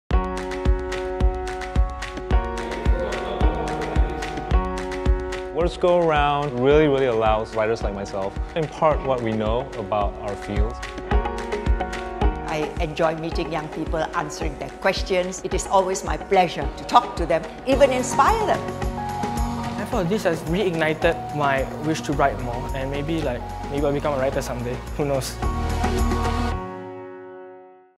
Words Go Round 2015 Open House
Words Go Round 2015 Open House with Huzir Sulaiman, Troy Chin, Dave Chua, Catherine Lim & Lee Thean-jeen.